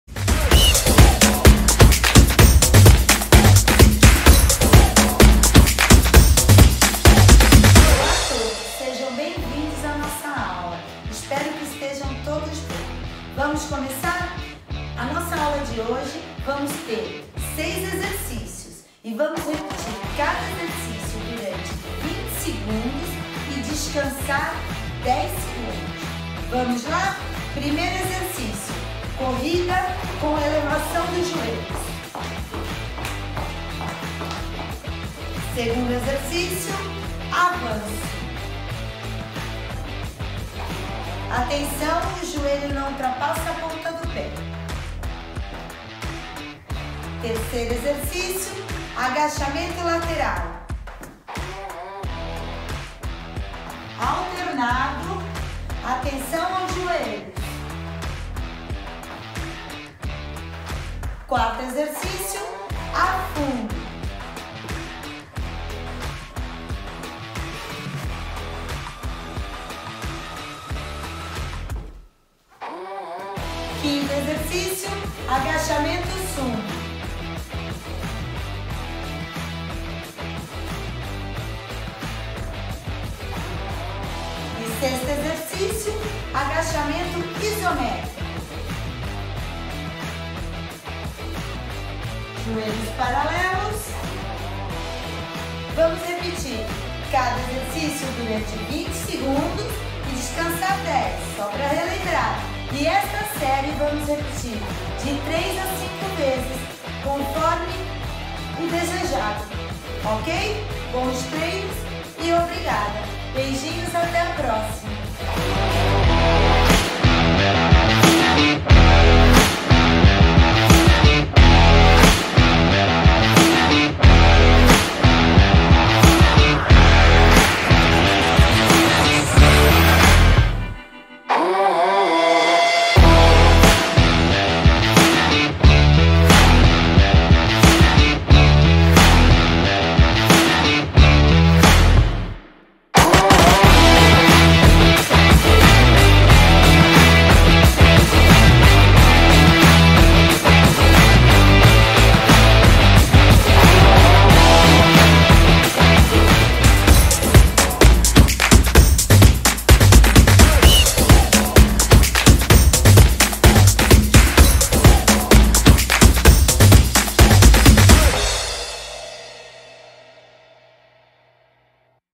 DESPORTO | DESPORTO PARA TODOS - VAGOS EM AÇÃO
No contexto atual de pandemia e atendendo à necessidade de nos mantermos todos em casa, é importante manter um nível adequado de atividade física.

Ciente desta importância, o Município de Vagos possibilita aos alunos do programa municipal Vagos em Ação e a todos os interessados, a possibilidade de manter uma prática regular de atividade física, de acordo com as orientações dos nossos monitores.

Fique em casa, mantenha-se seguro, mas mexa-se, pela sua saúde.